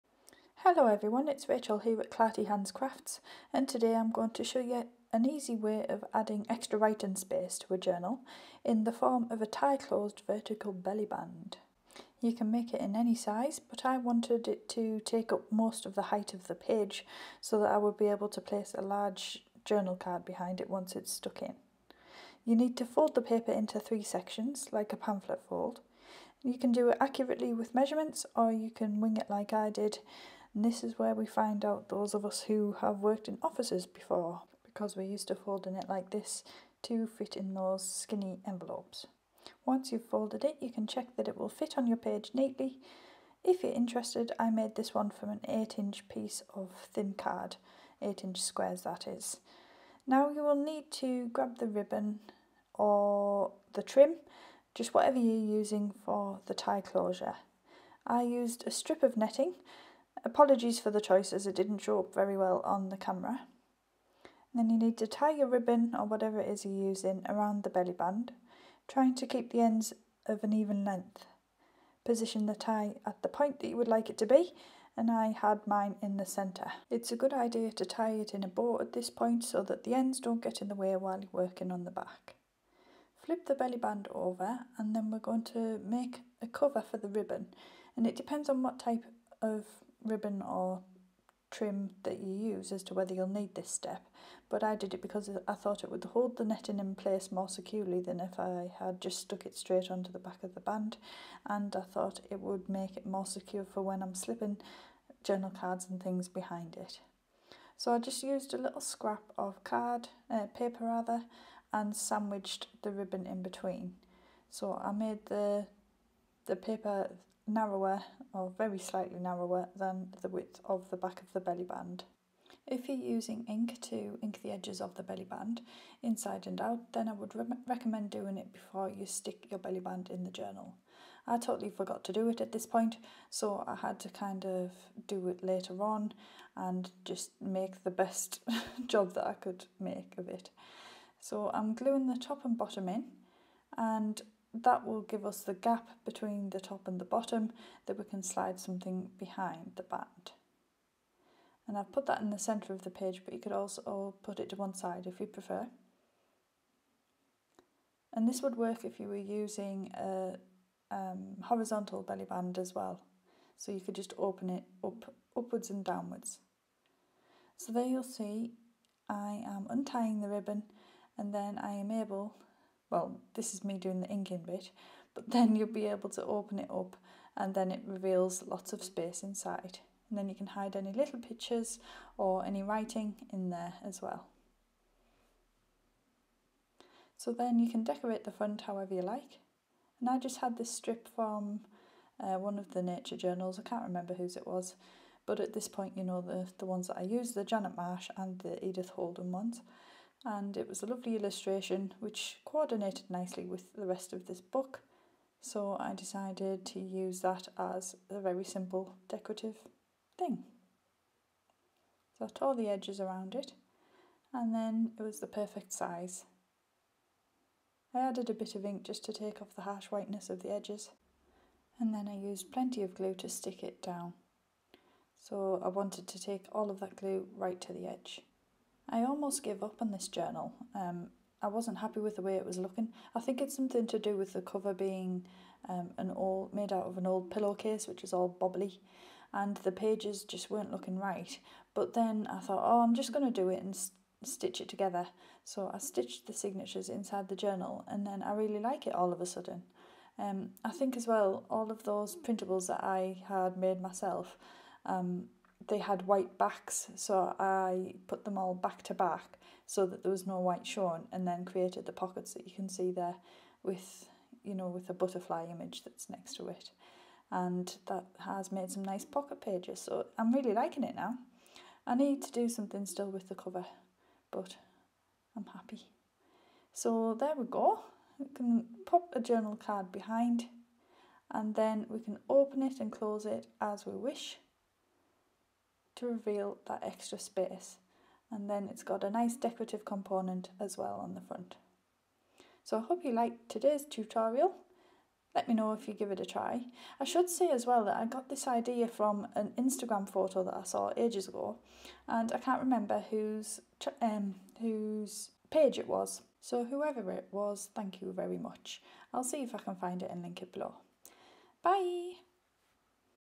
TIE CLOSED BELLY BAND WITH HIDDEN WRITING SPACE - Junk Journal Tutorials - Inspiration and Ideas
Today we’re creating a large tuck-spot and hidden writing space in one quick and simple project. It’s a vertical belly band with a ribbon tie closure.

My video kit:

My ring light
My camera
My microphone

Featured craft supplies:

Art Glitter Glue
Ultra Fine Metal Tip For Art Glitter Glue